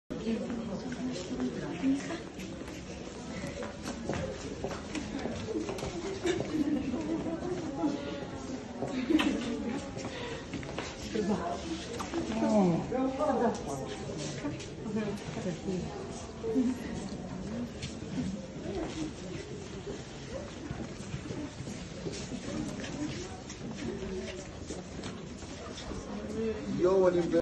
WATCH: 9-Year-Old Boy Held Hostage By Hamas Runs Into His Father's Arms After Being Freed
The Schneider Children's Medical Center has released a video of a 9-year-old boy held hostage by Hamas who has been freed reuniting with his father.